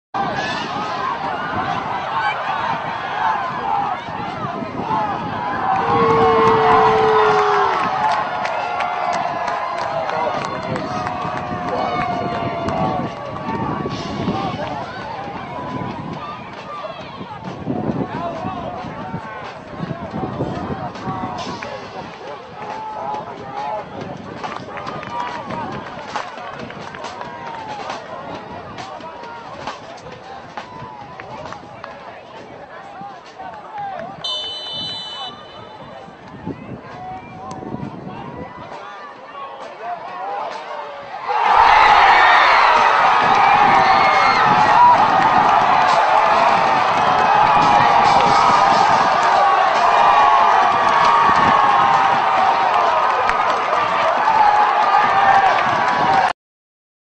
80 Yard Goal Ties State Soccer Championship Video
80 yards is an estimate, but this guy really boots this ball and puts it right past the goalie to tie up the score late in a championship game.